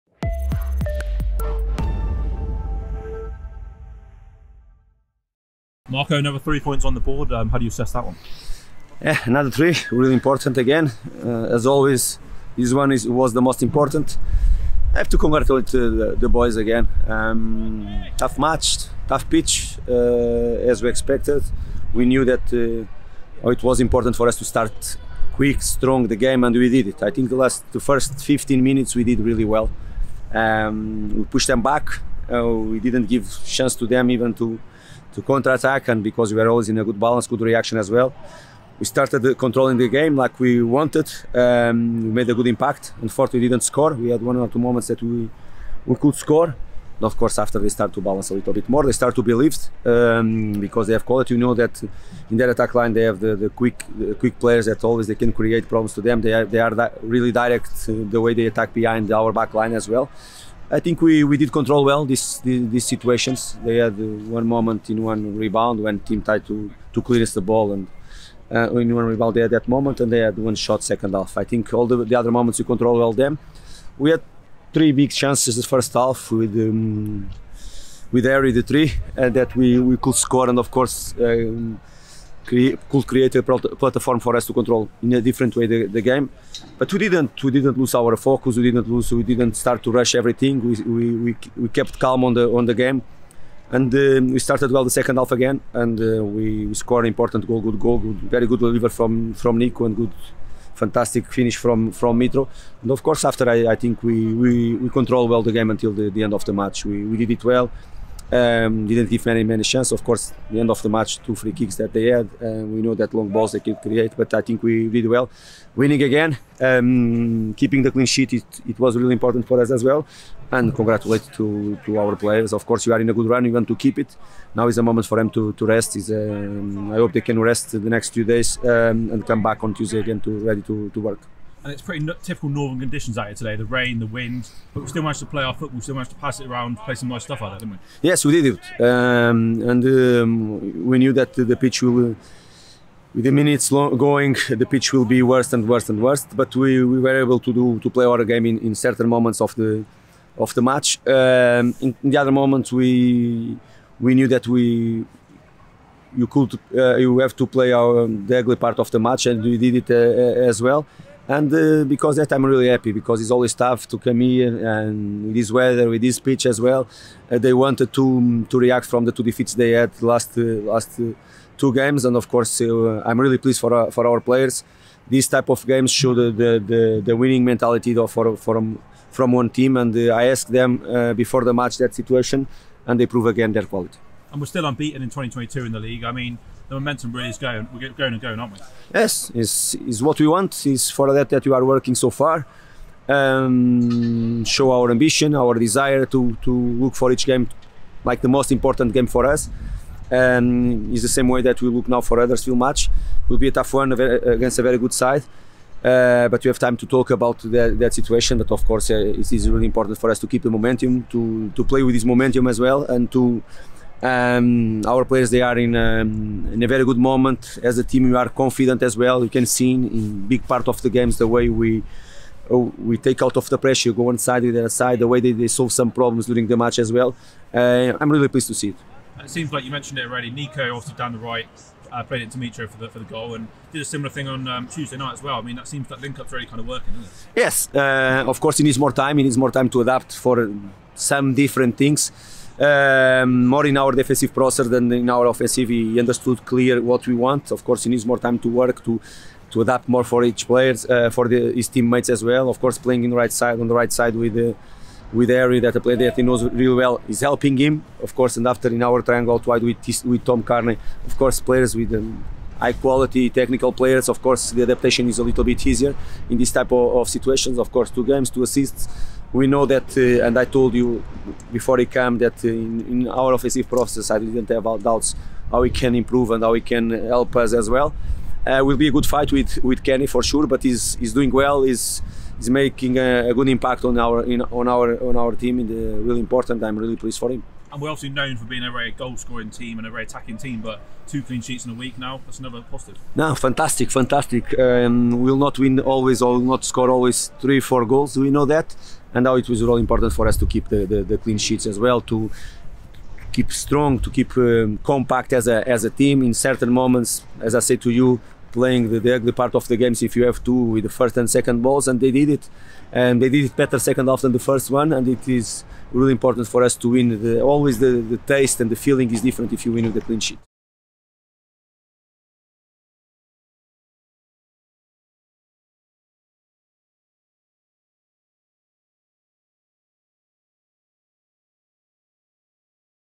Marco Silva: "We Controlled The Game Well" | Hull City Reaction
Marco Silva spoke to FFCtv as his side picked up another three points from a tricky contest in Hull.